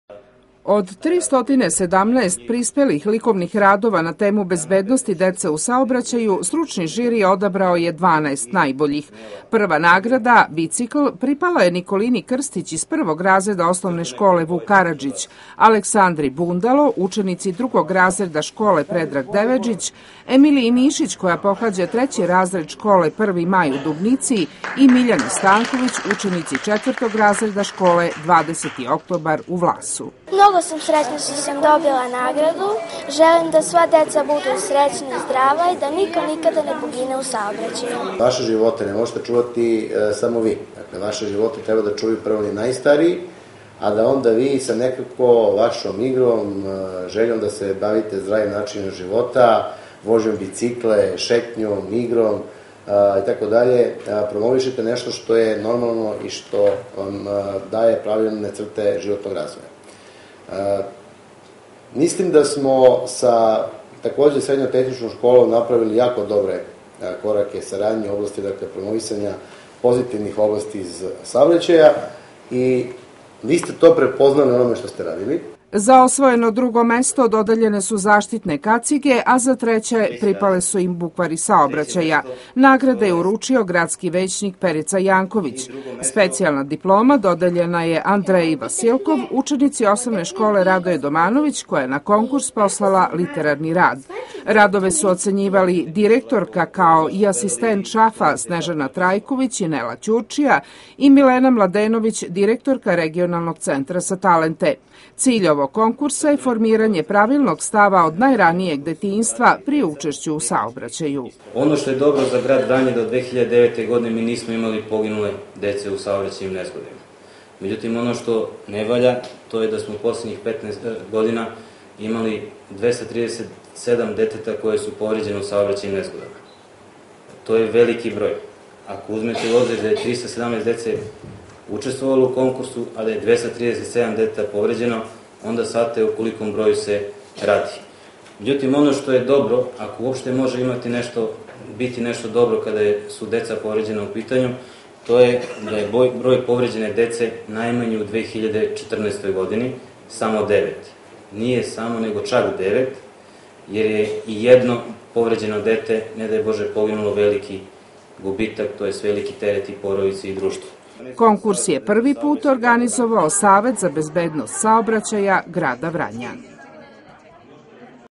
RTV Vranje Savet za bezbednost saobracaja Dodela nagrada 16 06 2015
urucene nagrade za najbolje radove na konkursu bezbedni smo ucesnici u saobracaju